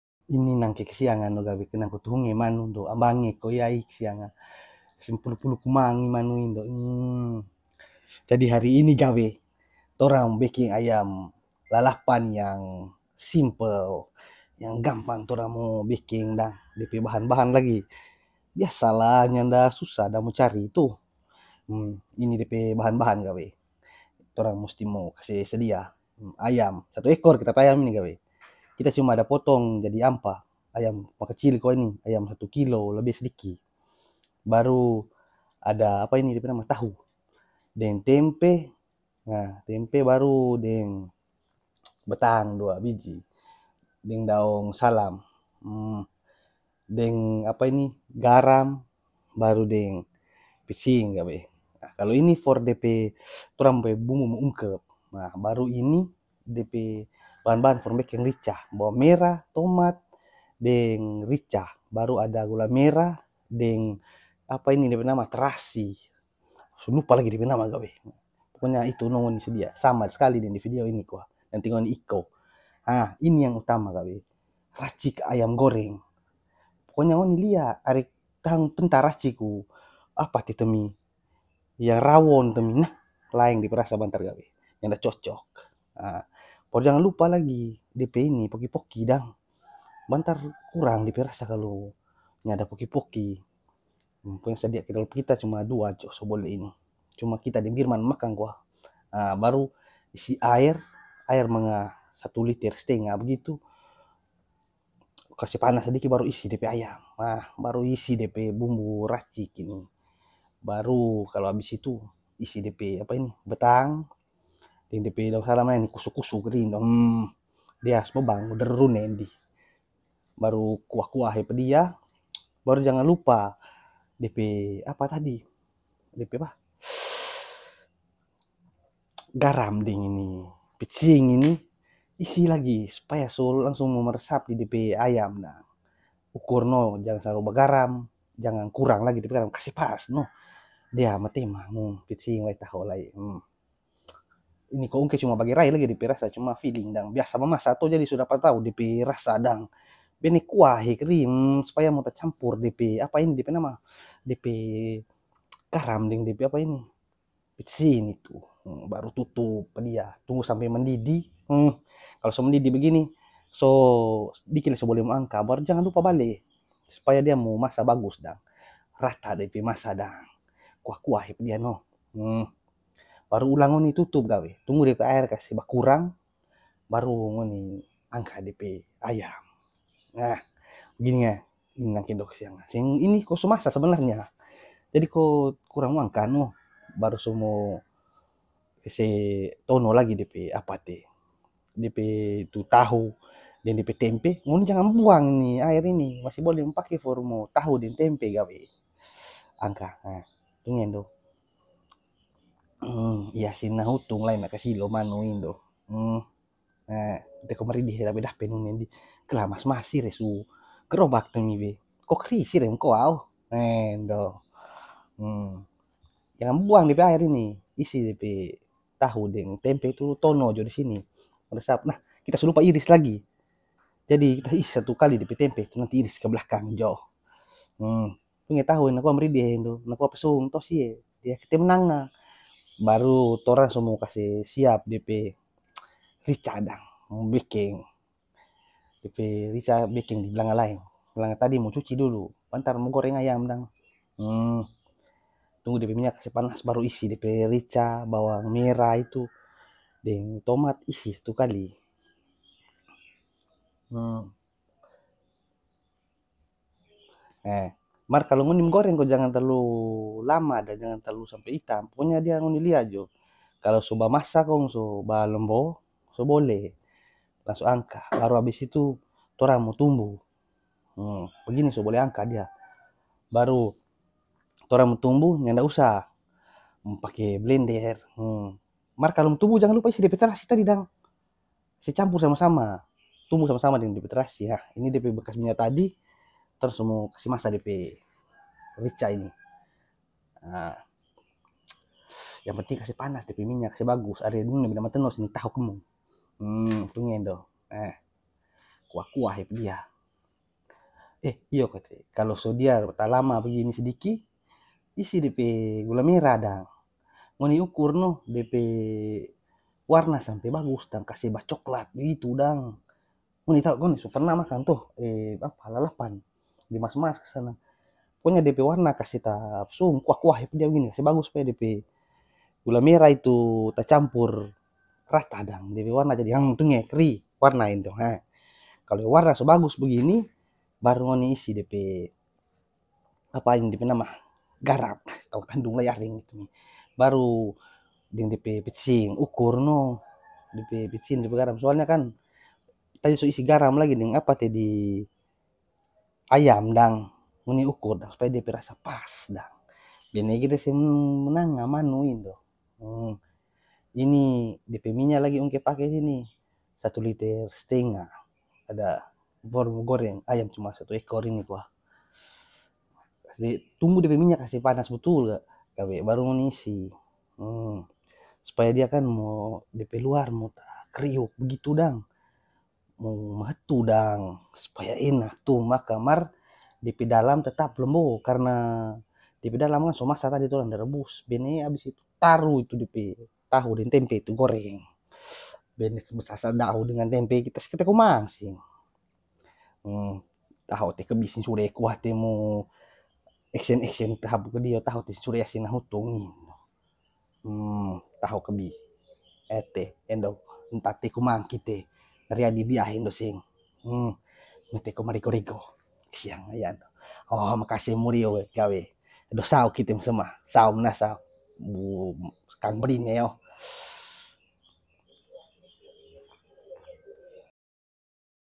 Ayam lalapan ekonomis
Hallo gawe
kali ini ungke mo bkng ayam lalapan ekonomis, depe bahan cma gampang so ada smua di video
ba uni sampe abis neh gawe 😁